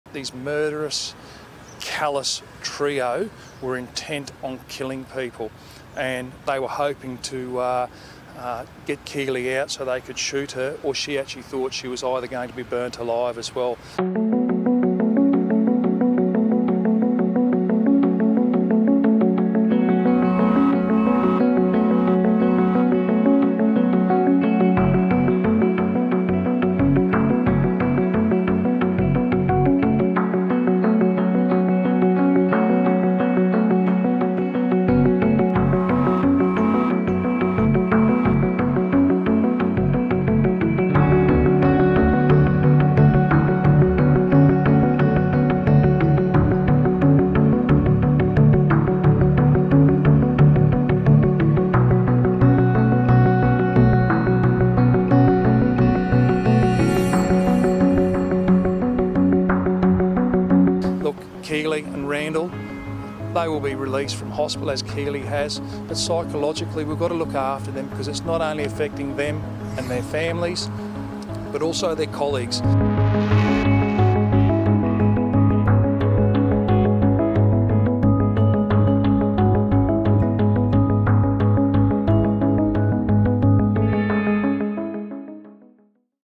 ‘Burnt alive’: Rookie cop’s haunting texts
Haunting new information has revealed the sick tactics the gunmen used to try and flush out a rookie cop after executing her co-workers in front of her. Full Story